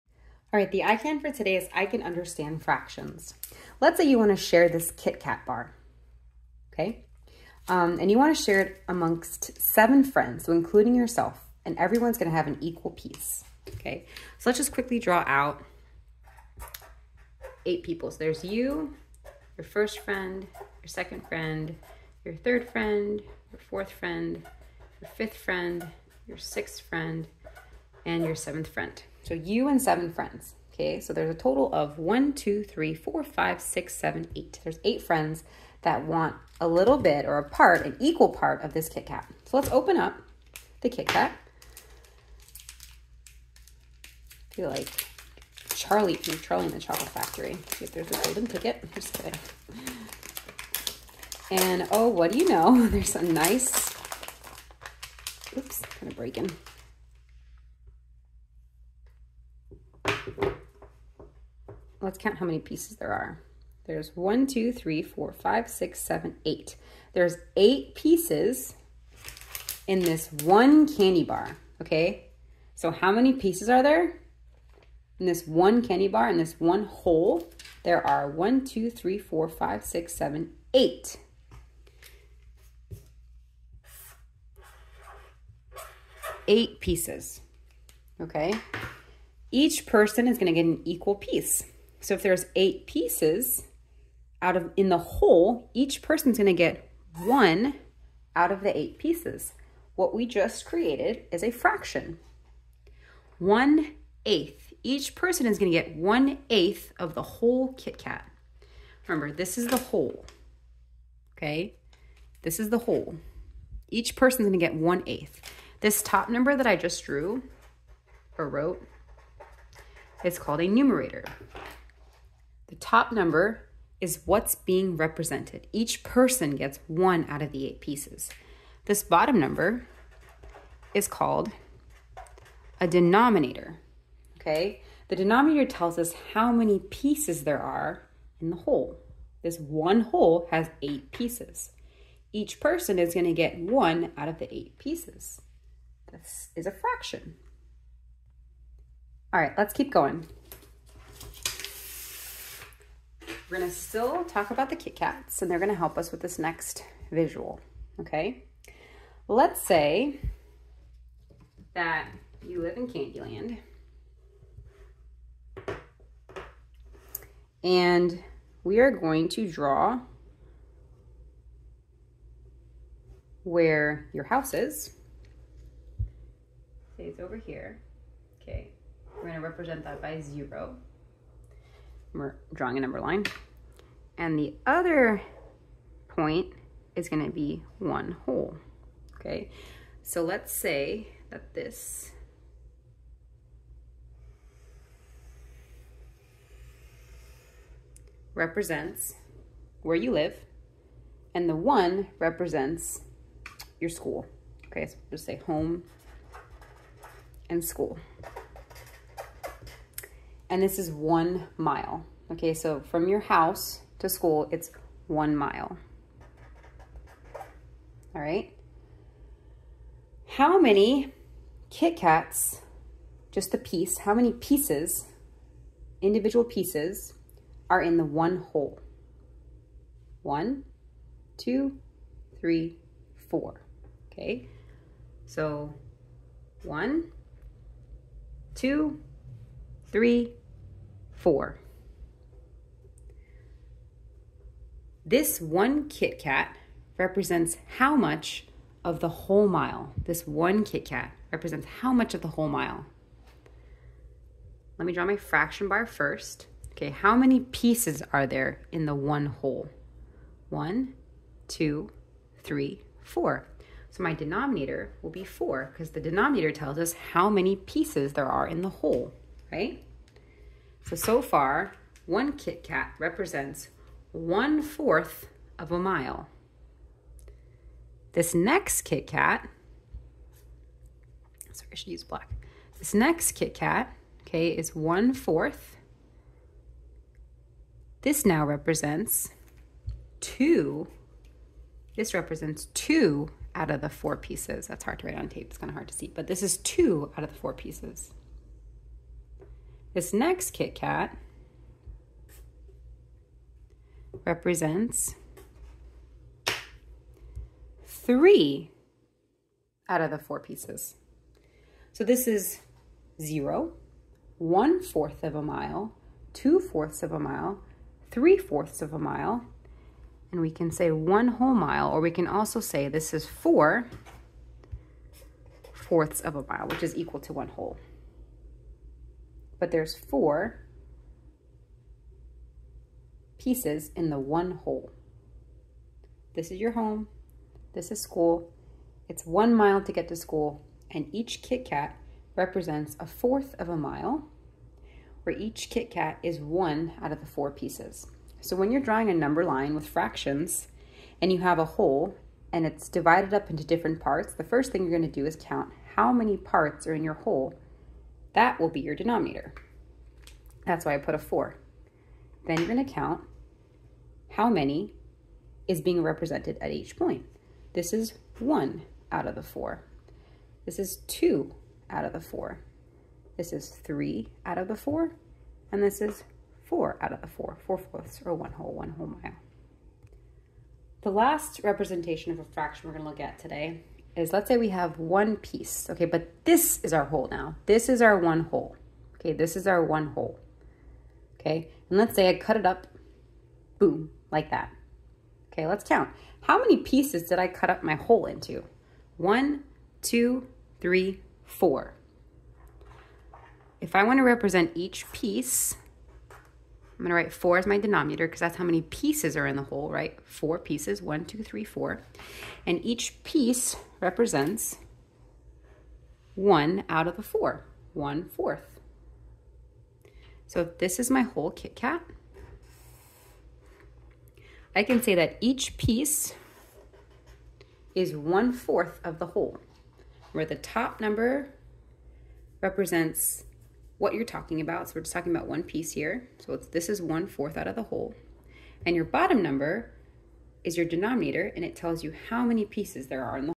3rd Lesson 9 Understand Fractions
MA.3.FR.1.1 Represent and interpret unit fractions in the form 1/n as the quantity formed by one part when a whole is partitioned into n equal parts.